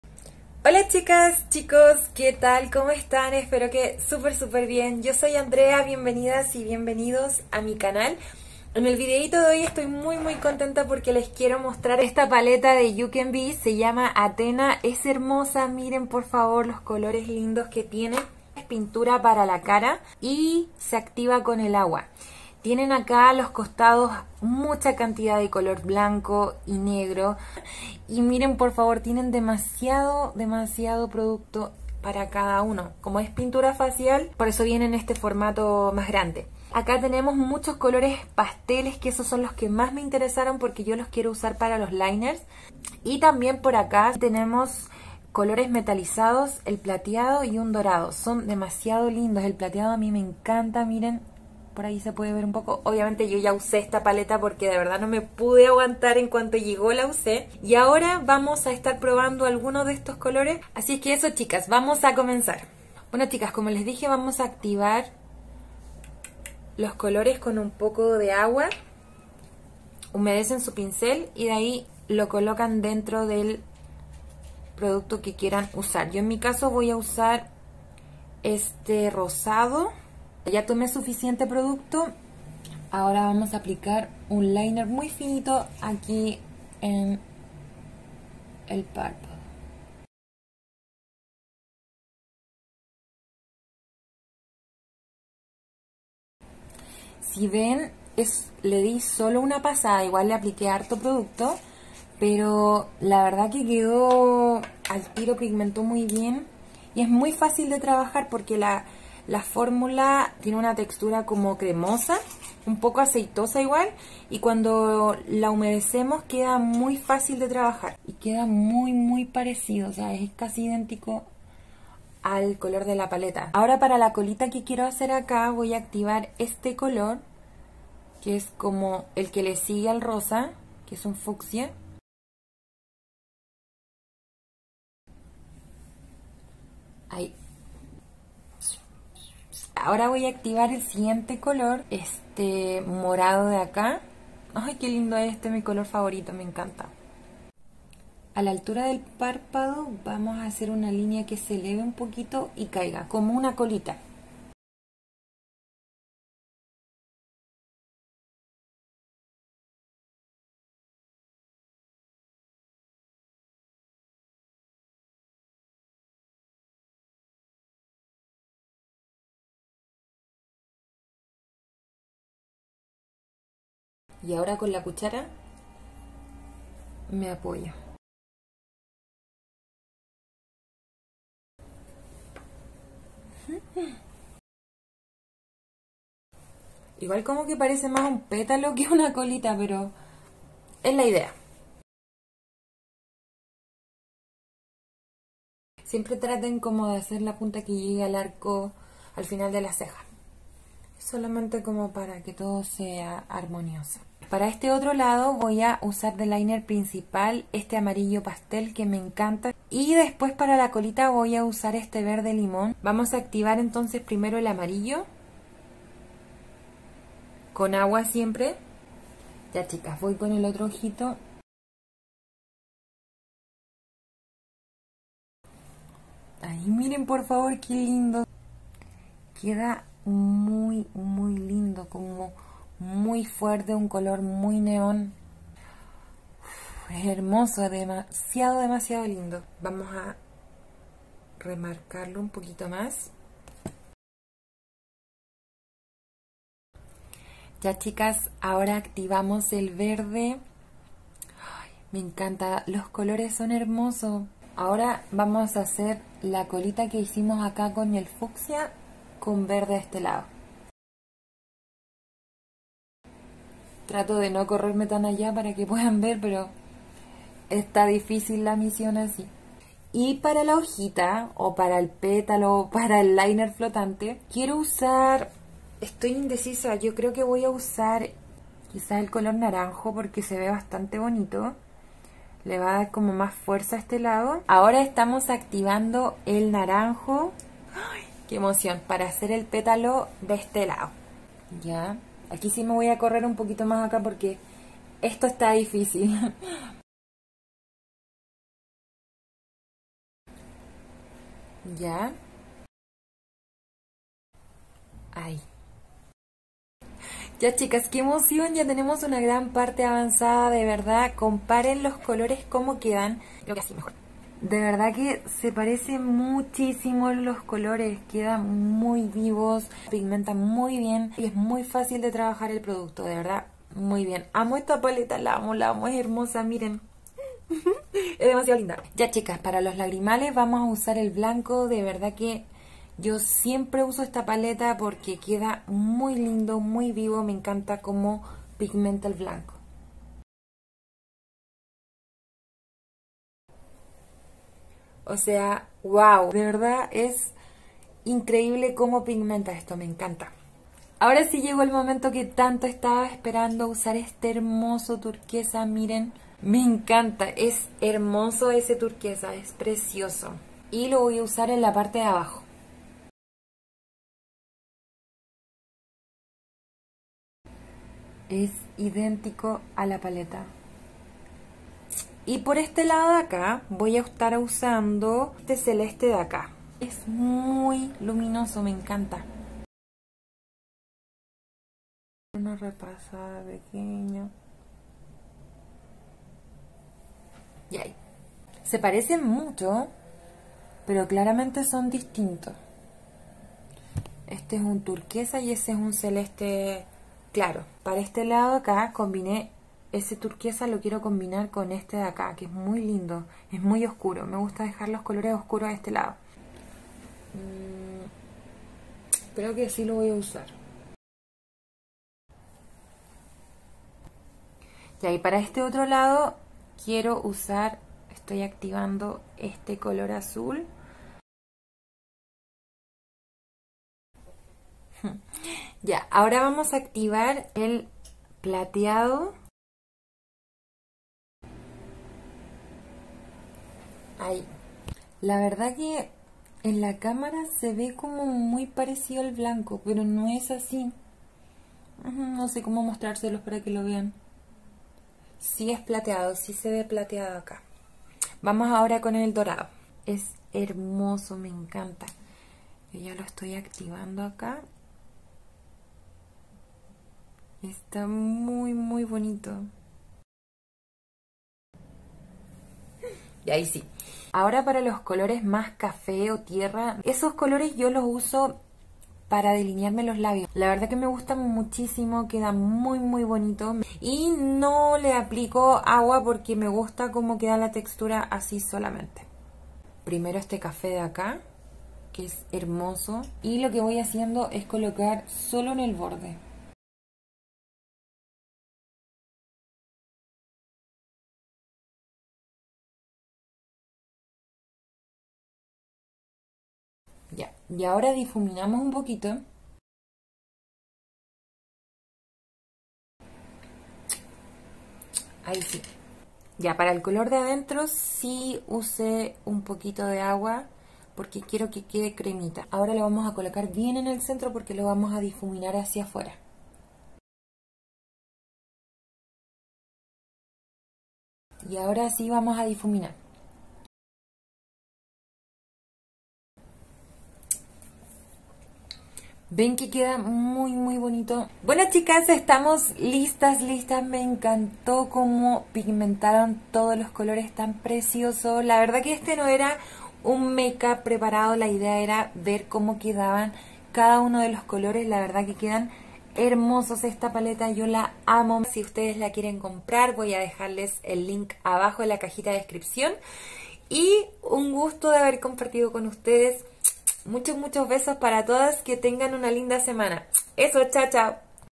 Probando la paleta Athena de UCANBE makeup face paint - Andrea
Hola Chicas cómo están??!! les quiero compartir esta reseña de la hermosa paleta Athena de UCANBE, que lo disfruten y prueben también! :)

Les dejo el link de amazon donde compré la paleta con los 10 pinceles: